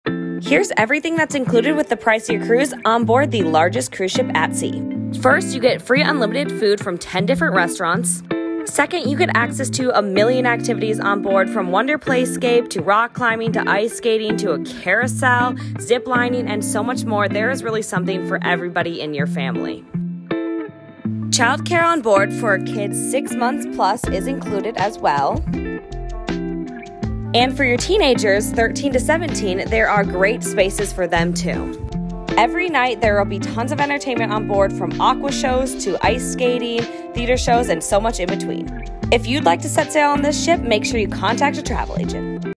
Everything That's Included on Your Royal Caribbean Cruise
Curious to know what's included with your cruise fare? Here's a rundown of all the free activities, entertainment, food, and more on board the largest cruise ship at sea!